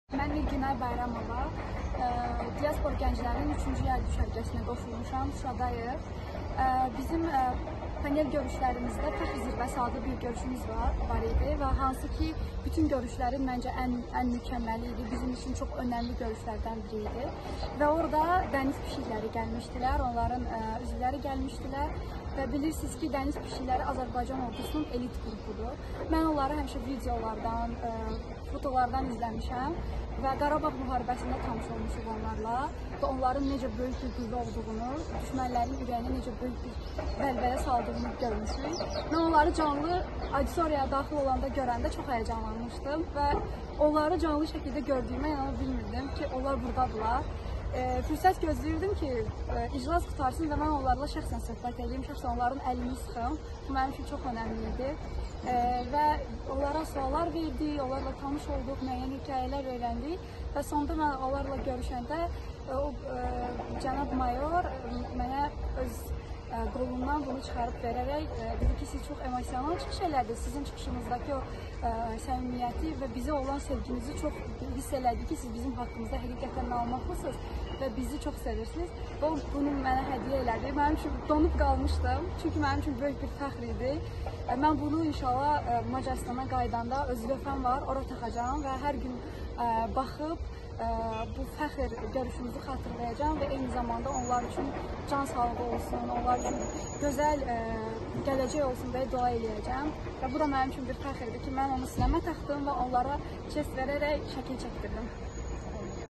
Düşərgə iştirakçısı Şuşada misilsiz hədiyyə alıb/ Участница лагеря получила в Шуше бесценный подарок
Diaspor Gənclərinin III Yay Düşərgəsinə Macarıstandam qatılan Mirgünay Bayramova qədim Şuşada aldığı misilsiz hədiyyə barədə təəssüratlarını bölüşüb.

Gənc soydaşımız Diaspor TV-yə müsahibəsində bildirib ki, “Fəxr Zirvəsi” mövzusunda təşkil edilən panel iclasını heç bir zaman unutmayacaq: “Bu, bizim üçün ən mükəmməl və ömənli görüşlərdən biri idi. Həmin görüşə Azərbaycan Ordusunun gizli qəhrəmanları – “Dəniz pişikləri”nin bir qrup üzvü gəlmişdi. Onları İkinci Qarabağ müharibəsindən tanıyırıq. Necə böyük qüvvə olduqlarını, düşmənin canına vəlvələ saldıqları barədə çox eşitmişdik”.

Yay düşərgəsinədək cəngavərlərimizi yalnız videolardan izlədiyini bildirən Mirgünay Bayramova qeyd edib ki, panel iclasının keçirildiyi məkanda “Dəniz pişikləri”nin zabitlərini görəndə çox həyəcanlanıb: “Gözlərimə inana bilmirdim. Onlarla tanış olduq, maraqlı hekayələrini dinlədik, suallarımıza cavablar aldıq. Panel iclasının qurtarmasını gözləyirdim ki, cəngavərlərimizə şəxsən təşəkkürümü bildirim”.

Gənc soydaşımız bildirib ki, iclasdan sonra mayor rütbəli cəngavərimizin formasındakı emblemini ona hədiyyə edməsi gözlənilməz olub: “Bildirdi ki, tədbirdə çıxışım zamanı “Dəniz pişikləri” barədə məlumatlı olmağım, bu qüvvələrə böyük sevgi ilə yanaşdığım hiss olunurdu. Mən sevincdən donub qalmışdım. Bu, mənim üçün böyük fəxr idi. Mən emblemi sinəmə taxdım, qəhrəmanlarımıza hərbi salam verərək birlikdə şəkil çəkdirdim. Macarıstana döndükdən sonra emblemi lövhəmə taxacağam. Hər zaman görüşümüzü xatırlayacaq və onlara dualar edəcəyəm”.

Участница III Летнего лагеря диаспорской молодежи из Венгрии Миргюнай Байрамова поделилась впечатлениями о подарке, полученном в древней Шуше.

В интервью Diaspor TV наша молодая соотечественница подчеркнула, что никогда не забудет панельного заседания на тему “Вершина гордости”: “Это была одна из лучших и важных для нас встреч. На ней присутствовали секретные герои азербайджанской армии – группа членов “морских котиков”. Мы знаем их со второй Карабахской войны. Много слышали о том, насколько большой силой они являются и какой страх наводят на врага”.

Отметив, что до летнего лагеря видела наших рыцарей лишь на видео, Миргюнай Байрамова сказала, что была очень взволнована, увидев офицеров “Морских котиков” там, где проводилось заседание: “Не могла поверить своим глазам. Мы познакомились с ними, послушали их интересные истории, получили ответы на свои вопросы. Ждала окончания панельного заседания, чтобы лично поблагодарить наших рыцарей”.

Наша молодая соотечественница сказала, что один из наших рыцарей в звании майора подарил ей эмблему со своей формы, что стало неожиданностью: “Он отметил, что в ходе моего выступления на мероприятии чувствовалось, что я имею представление о “морских котиках”, что питаю к этим силам большую симпатию. Я потеряла дар речи от радости. Это была большая гордость для меня. Я прикрепила эмблему к груди, отдала честь нашим героям и сфотографировалась с ними. По возвращении в Венгрию прикреплю эмблему к своей доске. Всегда буду помнить нашу встречу и молиться за них”.